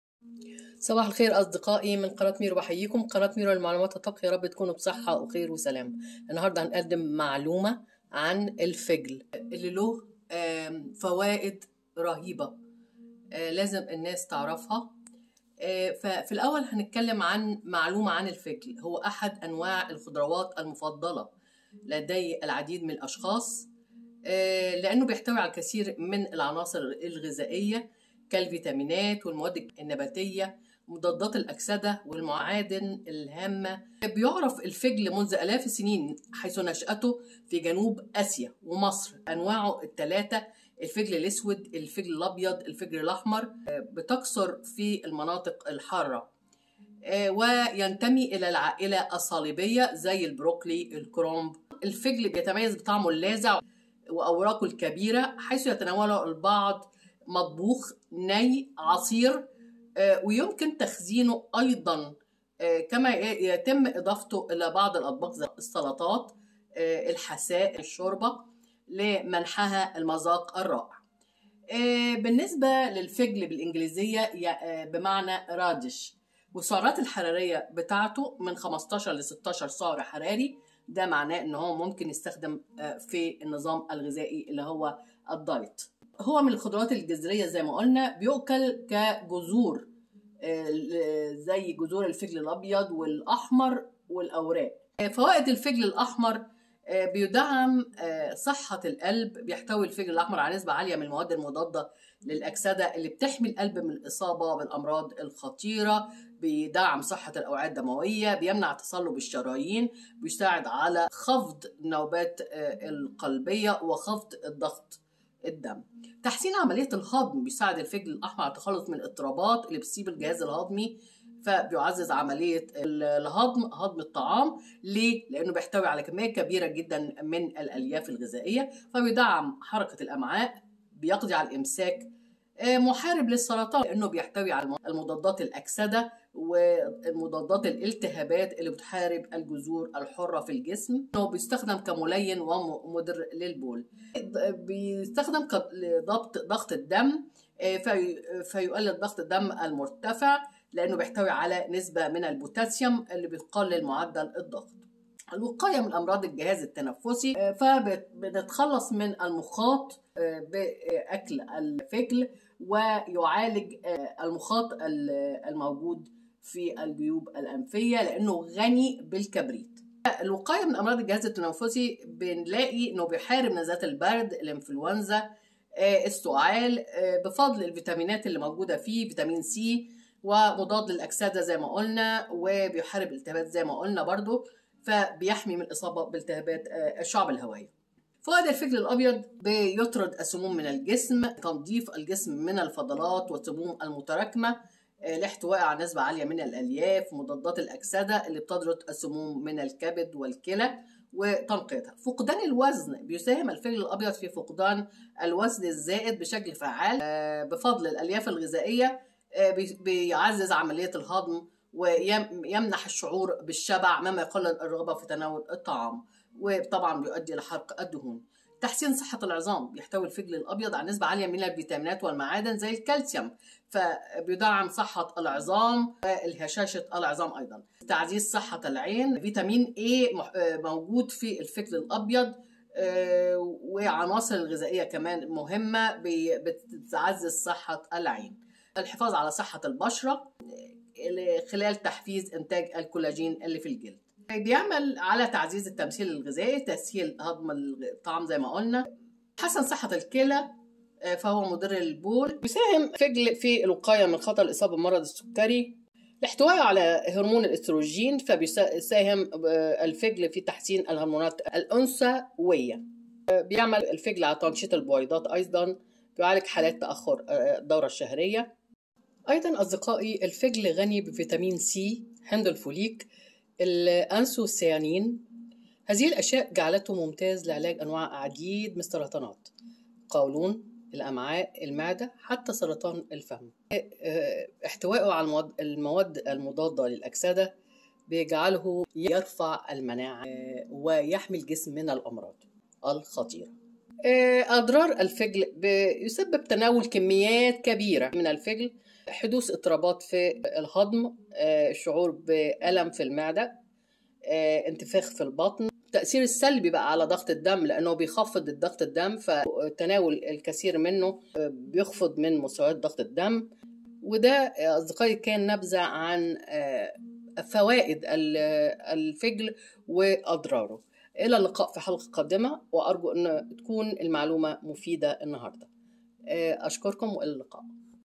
مالا نعرفه عن الفجل الاحمر او الابيض من فوائد او اضرار Radish
لفجل من الخضروات المهمه  لها جذور واوراق فى هذا الفيديو سنوضح اضرار الفجل عند تناوله بكثره وفوائد الفجل
Radish is an important vegetable that has roots and leaves. In this video, we will explain the harms of radishes when consumed in abundance, and the benefits of radishes.
More about this source textSource text required for additional translation information
Send feedback
Side panels